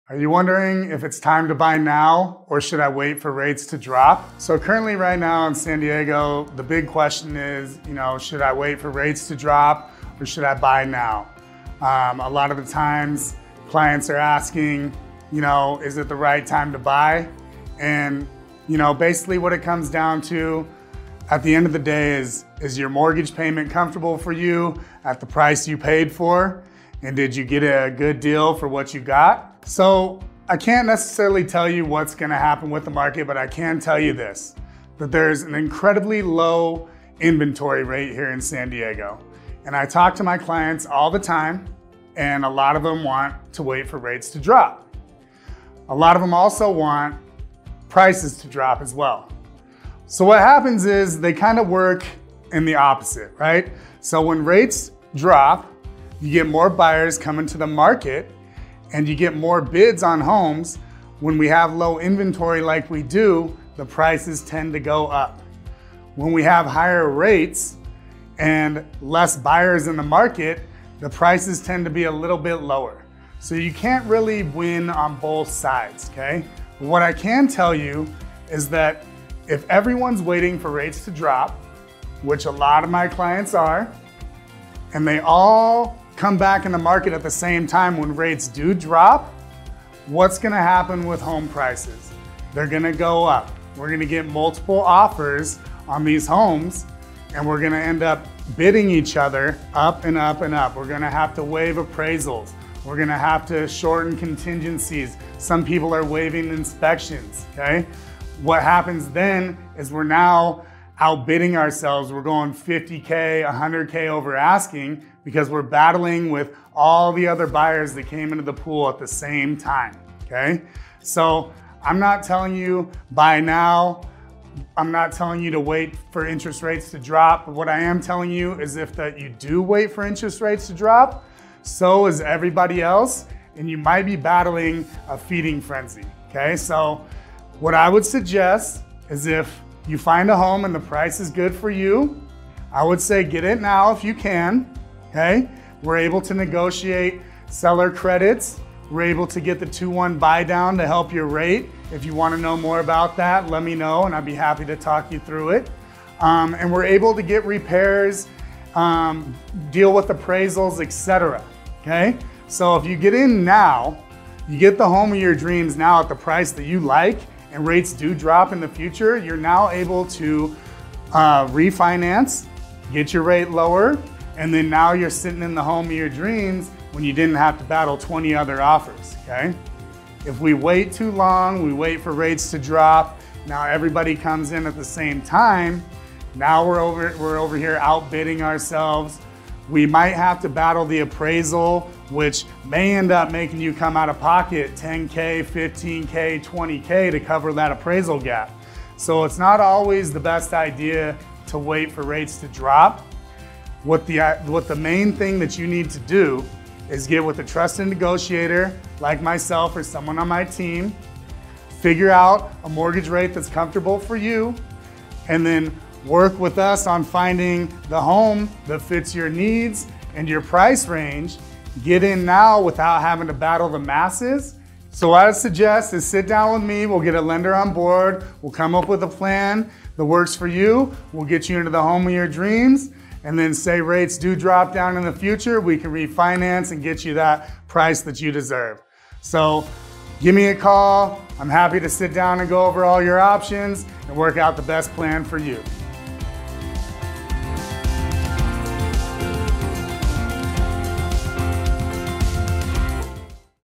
Relationship of Interest Rates and Inventory
In this video, San Diego Realtor Sean Setzer explores the important relationship between interest rates and real estate inventory. Understanding this relationship is critical for anyone looking to buy or sell real estate, as it can have a significant impact on the market.

He will discuss how changes in interest rates can affect inventory levels, as well as the buying and selling behavior of individuals and businesses. Additionally, He will provide some tips and strategies for navigating the real estate market during different interest rate environments.

Whether you're a seasoned real estate investor or just getting started, this video will provide valuable insights into the complex interplay between interest rates and inventory in the real estate market.

Kyle Whissel
eXp Realty of California, Inc. - DRE# 01878277
3180 University Ave, Suite 600